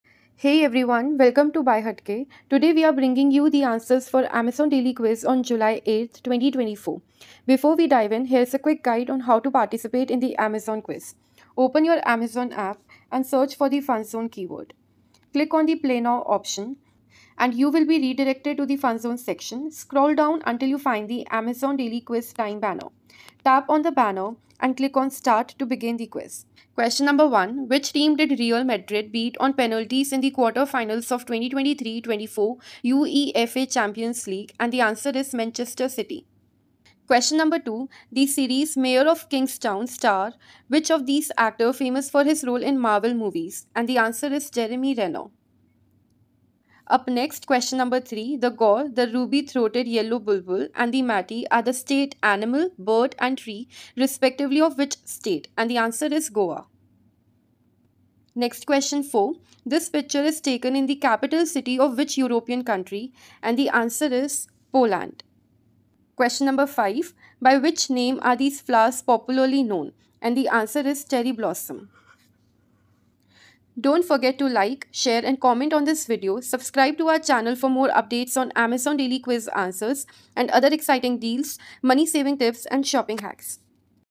Amazon Daily Quiz | Amazon Daily Quiz Time Answers Today | (08/July/2024)
In this amazon funzone quiz answers video - we will show you how to participate in amazon daily quiz and provide you the answers for amazon quiz today to win exciting prizes!
Today Amazon Daily Quiz answers
Daily quiz
Time stamps
0:00 Introduction
0:10 How to participate in the quiz
0:27 Question 1
0:38 Question 2
0:48 Question 3
0:59 Question 4
1:07 Question 5
1:14 Subscribe note